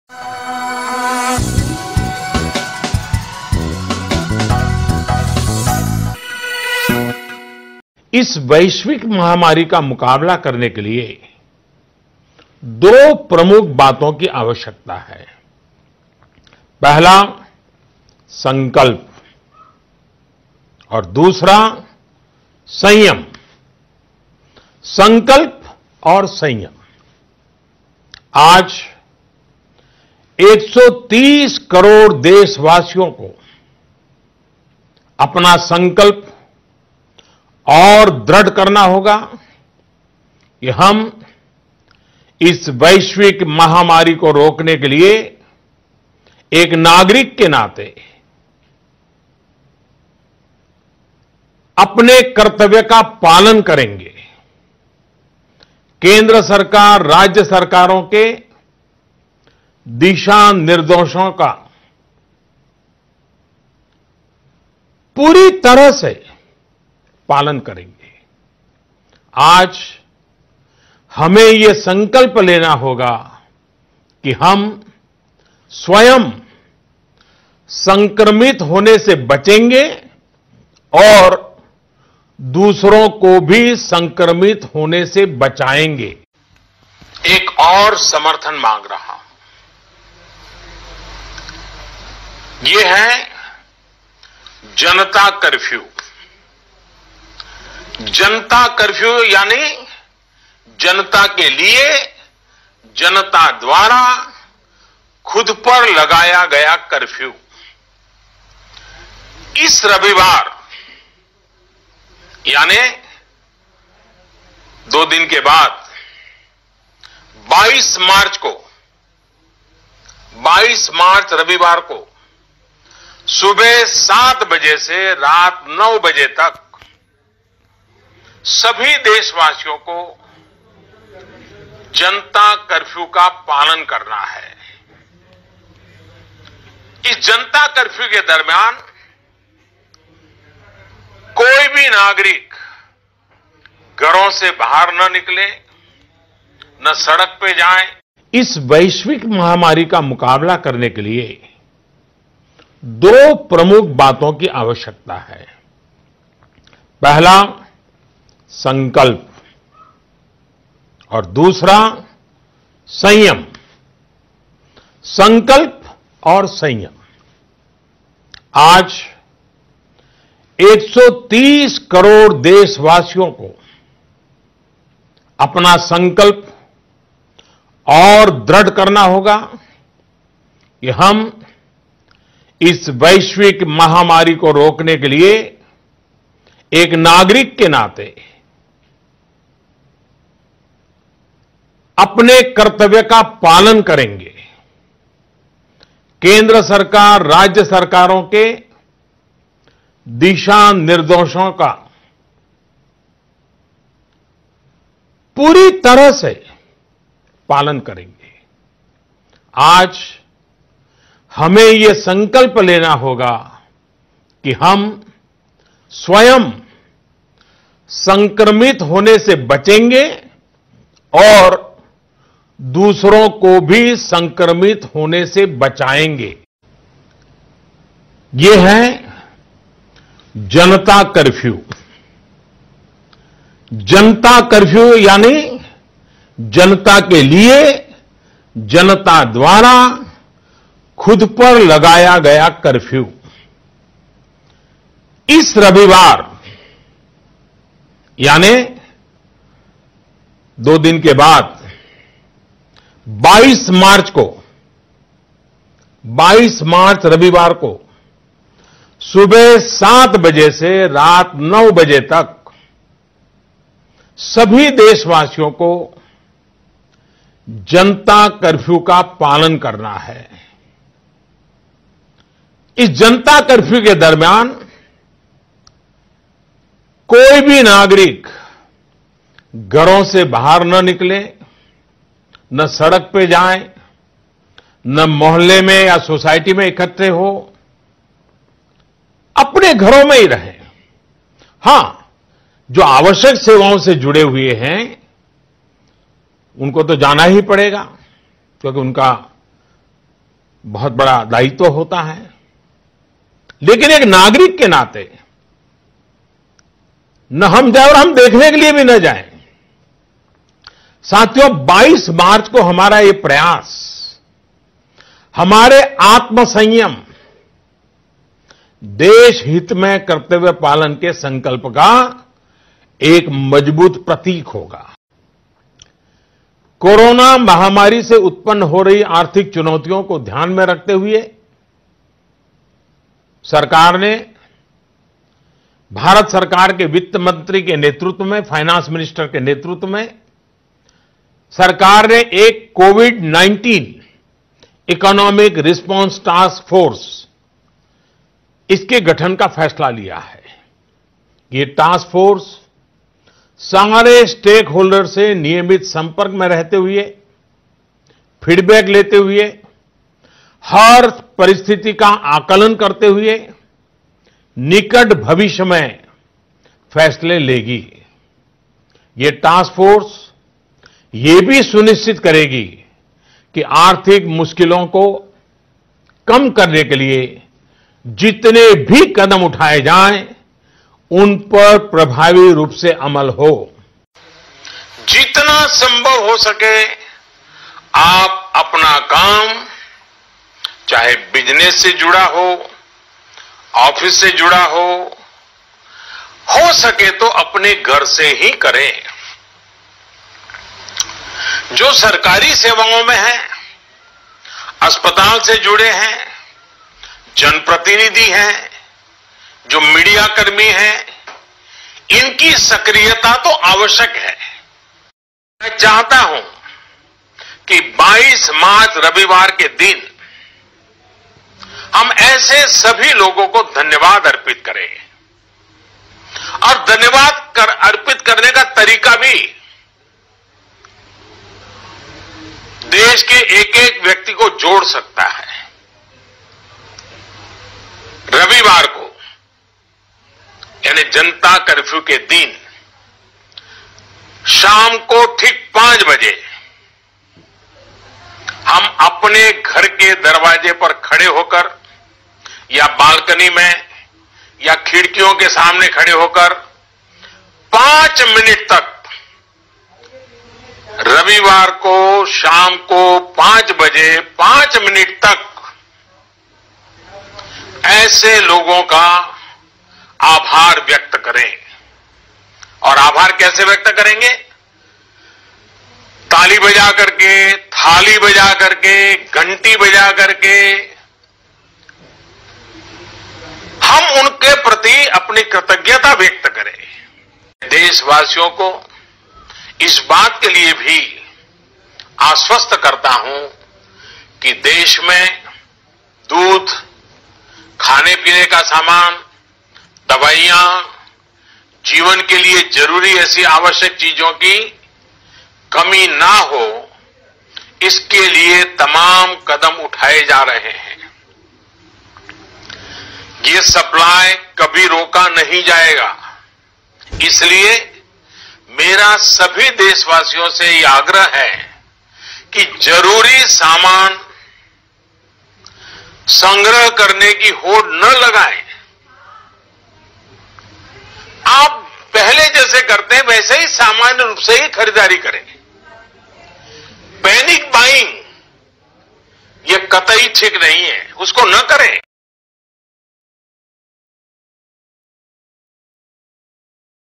Coronavirus (COVID-19): PM Modi Announces Janta Curfew On 22nd March from 7AM to 9PM, Don't step out
Coronavirus (COVID-19): PM Modi Announces Janta Curfew On 22nd March from 7 AM to 9 PM, Don't step out